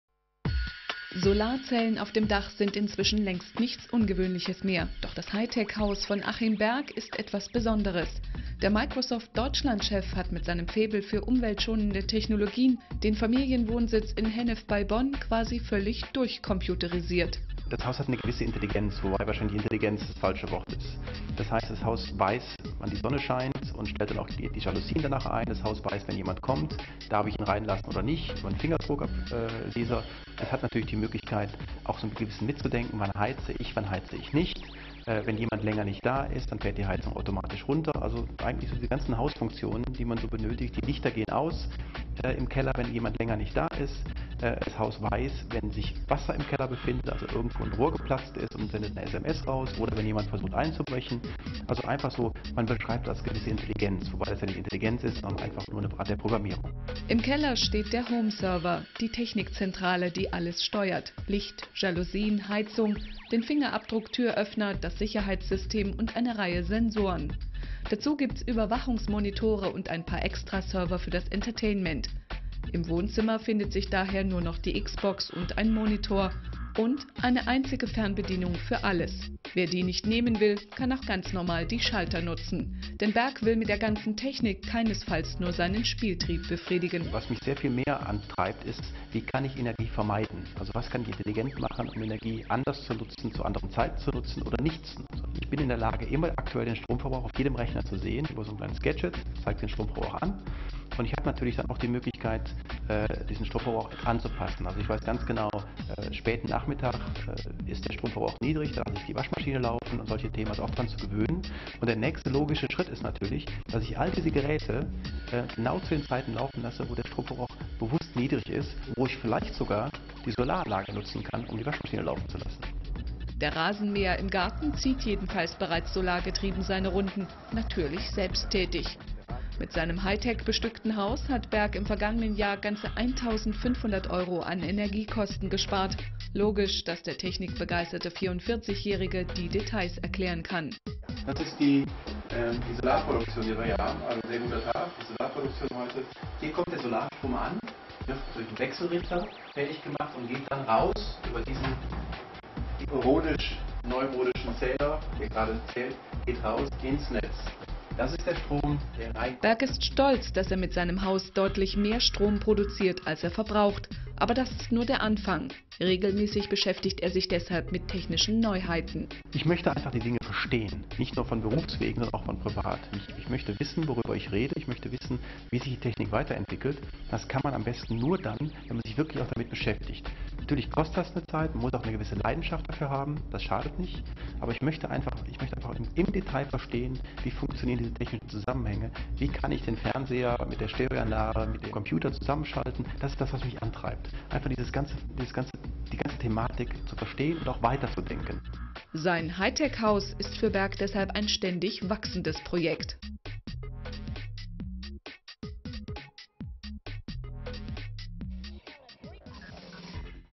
Das vollelektronische Haus des deutschen Microsoft-Chefs
Als habe man einem Star-Trek-Fan eine Kreditkarte in die Hand gedrückt und gesagt: "Mach, was Du willst!". Das Haus von Achim Berg, Microsoft-Deutschland-Chef, ist komplett durchcomputerisiert. Selbst der Rasenmäher fährt selbsttätig. FTD.de zeigt einen Streifzug durch das Hightech-Haus - das vor allem energiesparend ist.